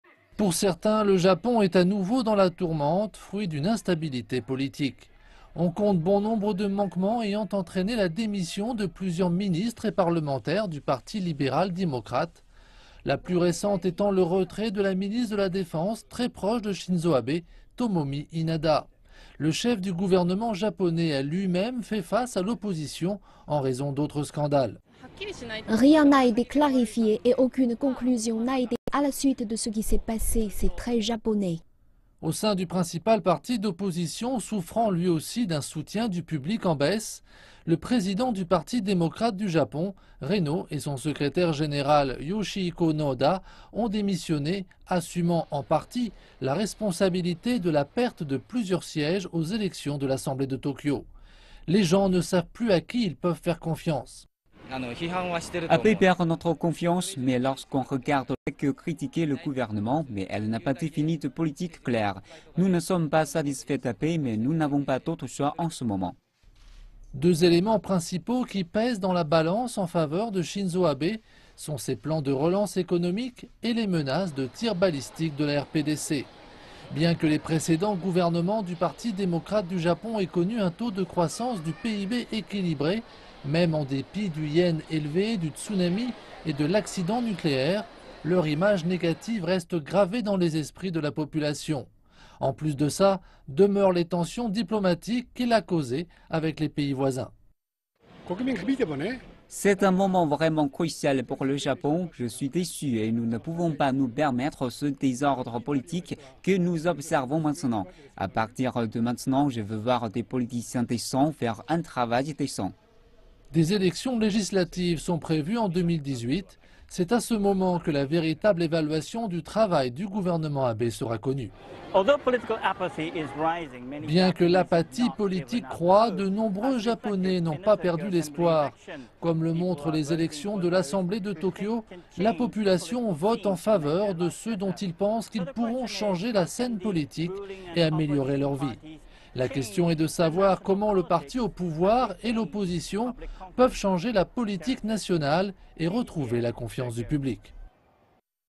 La démission de la ministre japonaise de la Défense, un nouveau coup dur pour Shinzō Abe
La ministre japonaise de la Défense a démissionné vendredi à la suite d’un scandale de non-divulgation de documents relatifs à une mission des forces d’autodéfense. Le Premier ministre japonais, Shinzō Abe, a également présenté ses excuses pour les actions de sa ministre et sur la façon dont le scandale a été traité.